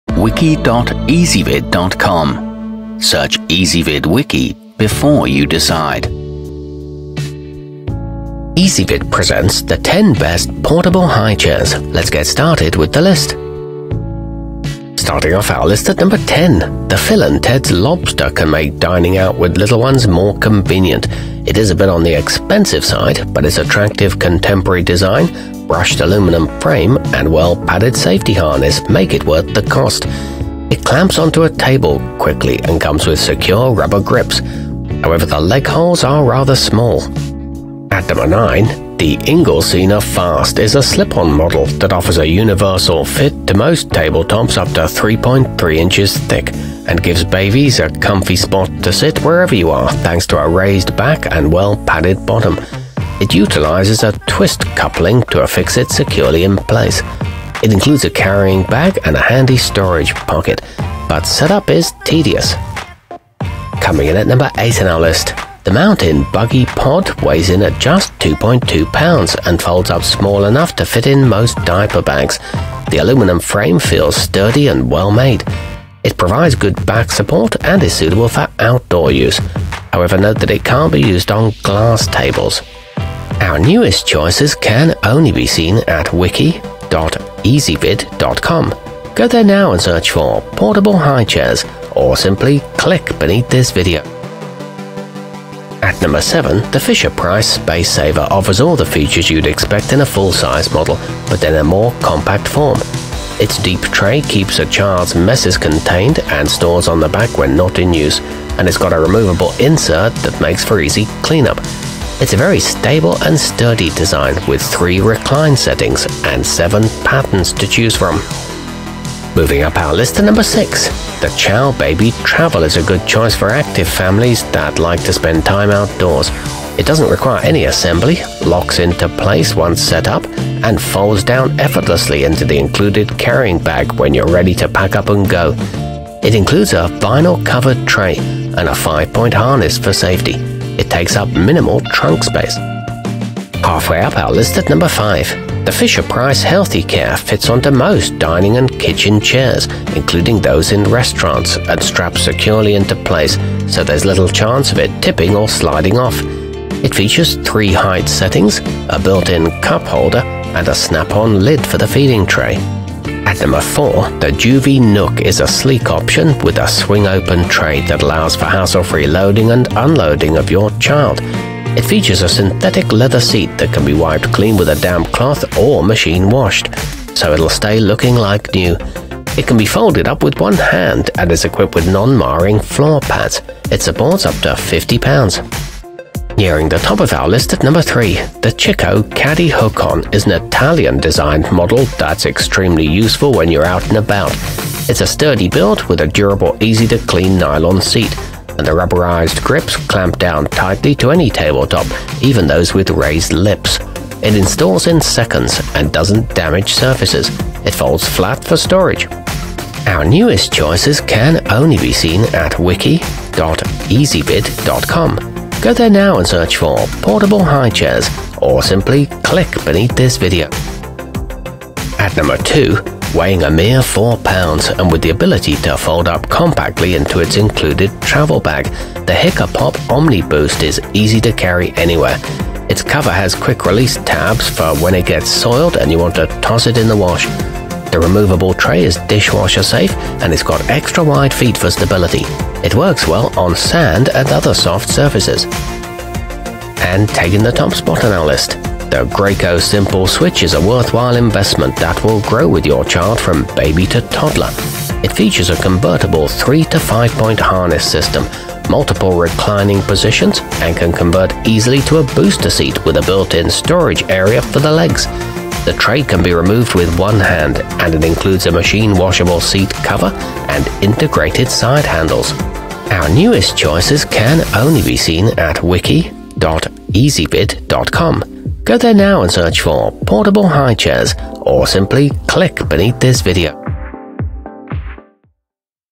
10 Best Portable High Chairs 2020
Our complete review, including our selection for the year's best portable high chair, is exclusively available on Ezvid Wiki.

Portable high chairs included in this wiki include the fisher-price healthy care, joovy nook, inglesina fast, ciao! baby travel, fisher-price spacesaver, hiccapop omniboost, mountain buggy pod, phil & teds lobster, chicco caddy hook on, and graco simpleswitch.

Portable high chairs are also commonly known as travel high chairs and folding high chairs.